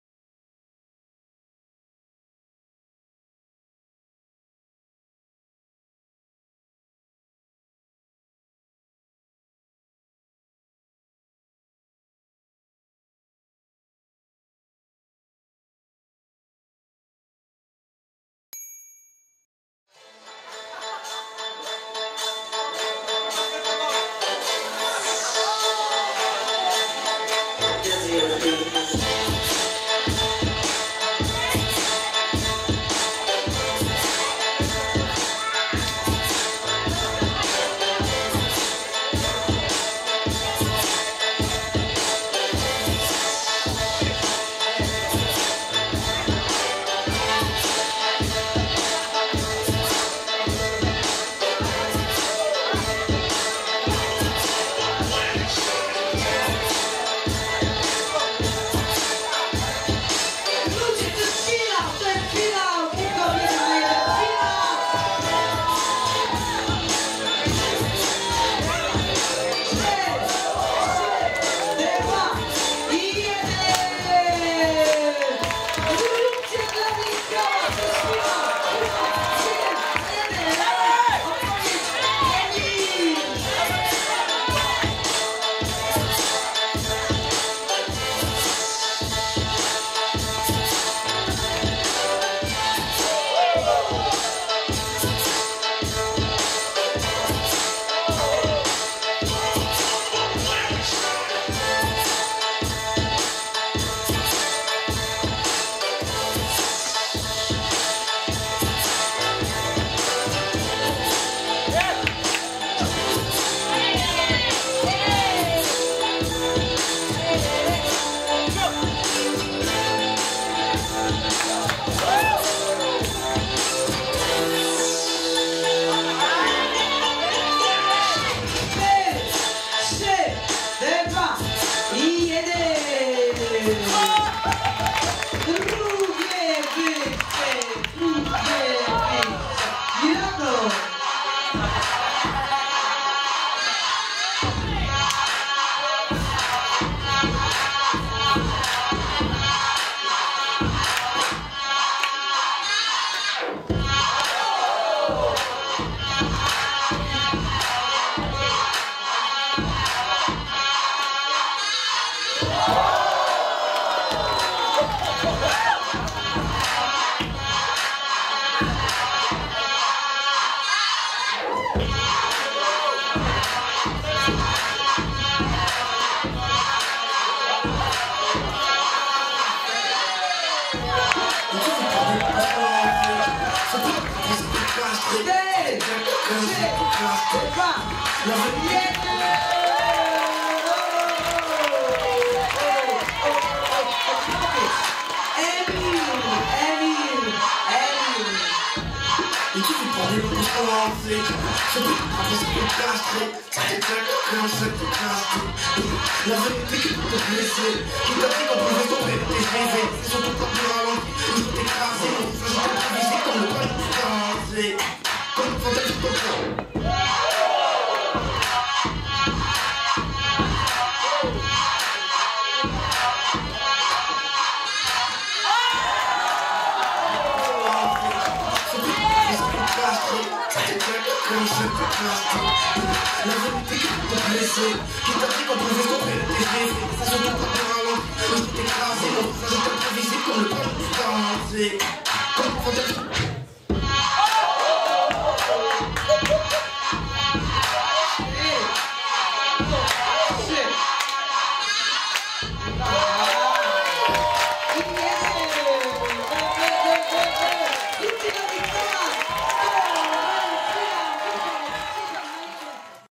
Finał Hip-Hop 1vs1 - Emir vs Miracle | Groove Contest 2013
Groove Contest 2013 - Poznań

Finał Hip-Hop 1vs1

Emir vs Miracle

Winner: Miracle